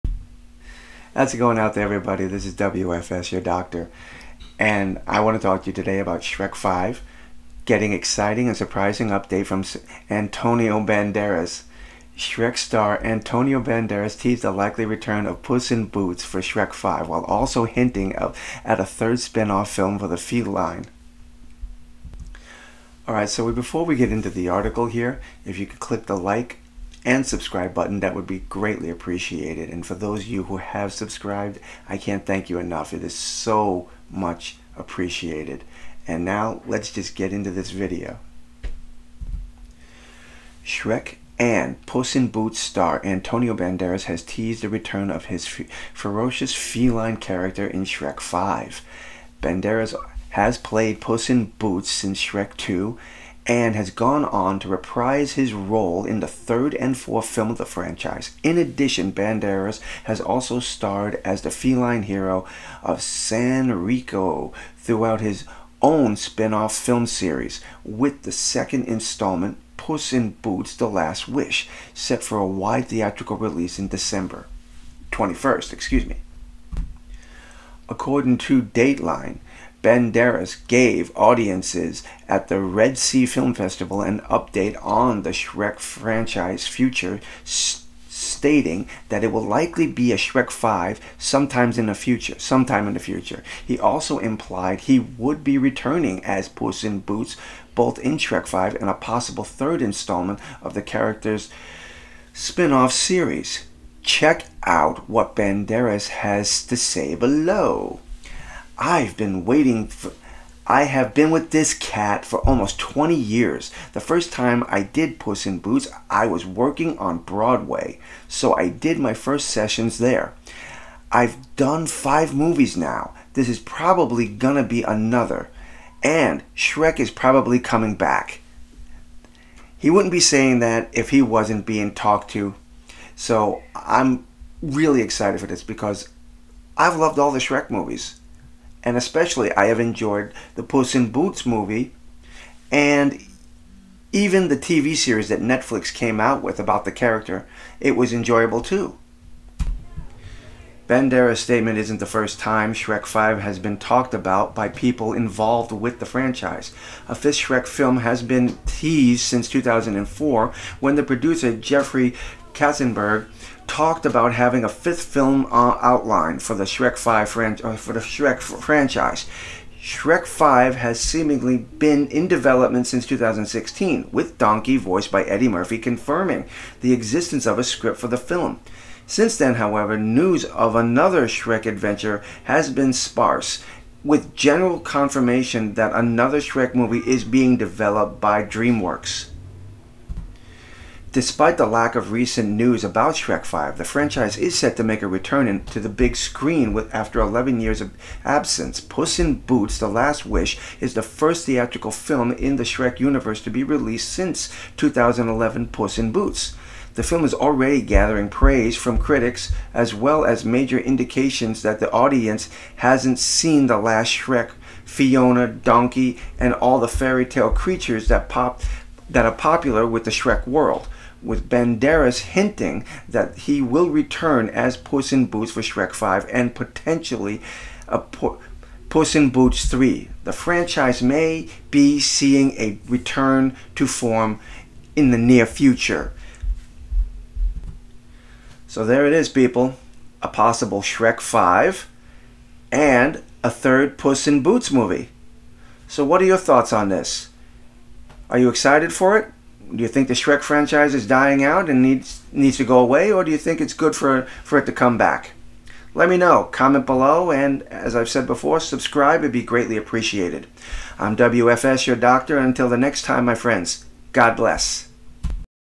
SHERK 5 ANTONIO BANDERAS EXCITING NEWS !
Antonio Banderas says Puss in Boots will ride again ! Yes Puss in Boots is on his way back to the big screen in his new movie Puss in Boots The Last Wish and while promoting the movie Antonio told fans that that he will be back in Sherk5 ! Shrek has not been seen on the screen since 2004 and fans would love to welcome the big ogre back along with all his friends !